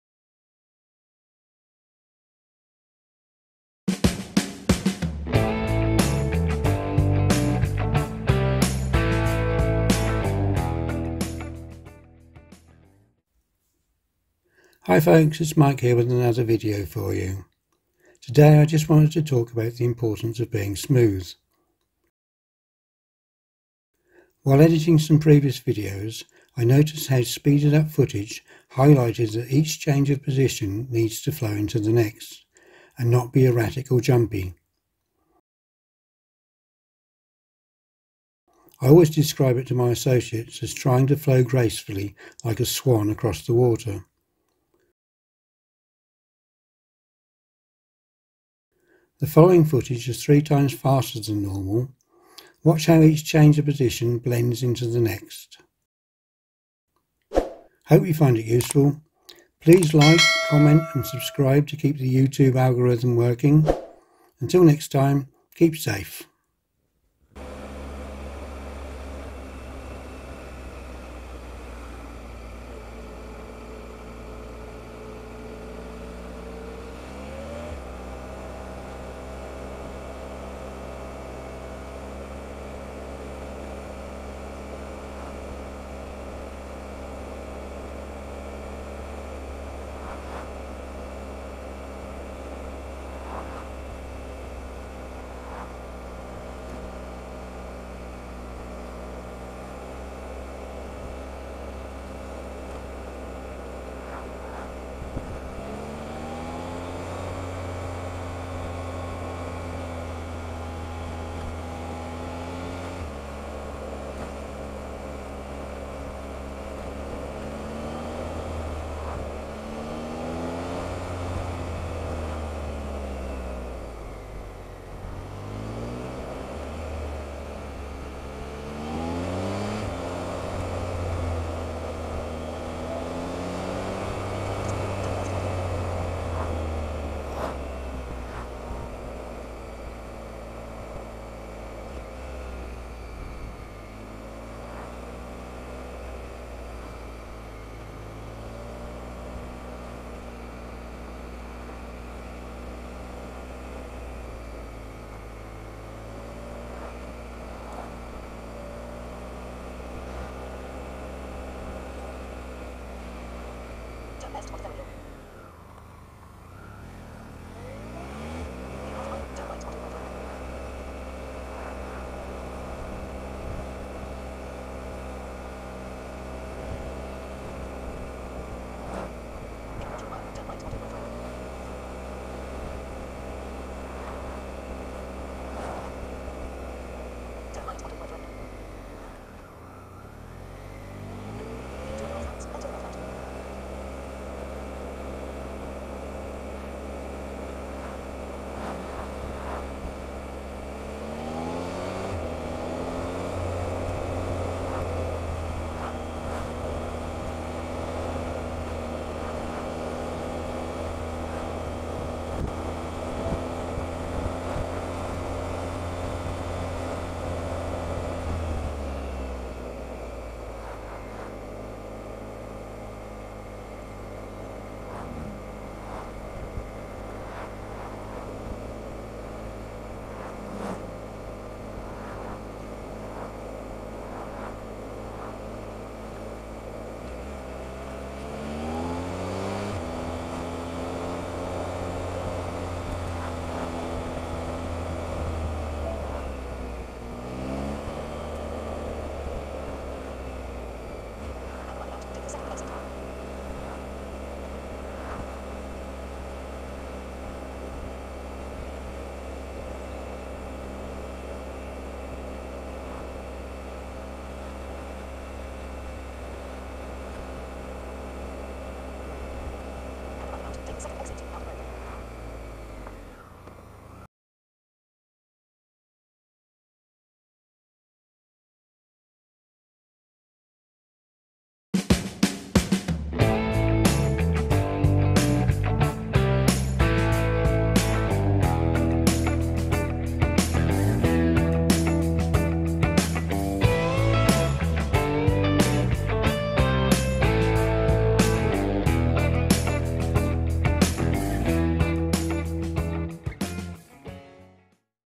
Smooth Flowing Lines | Motorcycle Riding on UK B Roads
This riding footage has been speeded up x3. Effectively giving riding speeds of 0 to 180mph, to display the smooth way that road position should blend and not jump or be jerky.